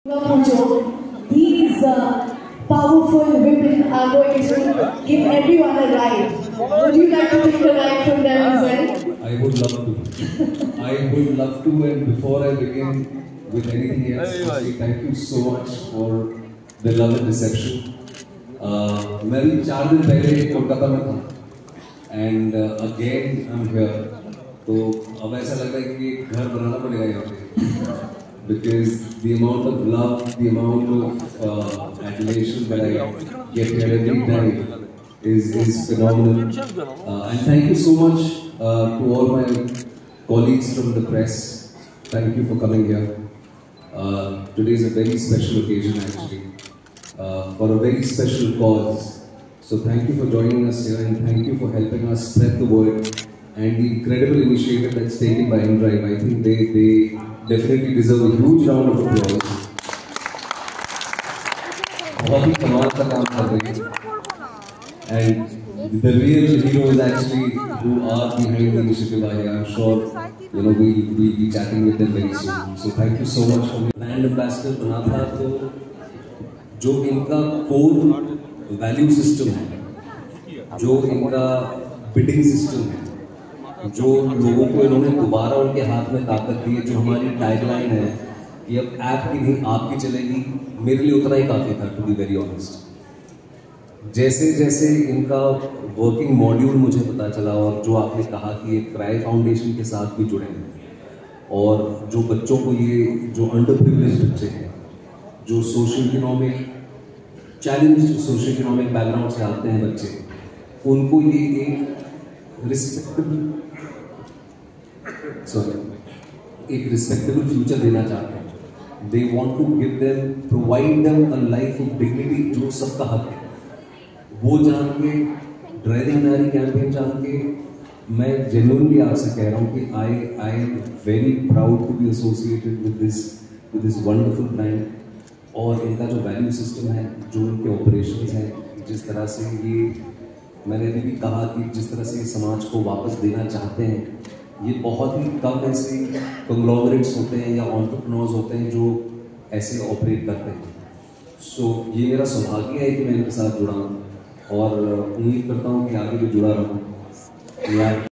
Vikrant Massey Praises inDrive's Driving Naari Initiative
In collaboration with the Rakshak Foundation Kolkata, inDrive, a global platform for urban services & mobility, announced the Driving Naari program in Kolkata.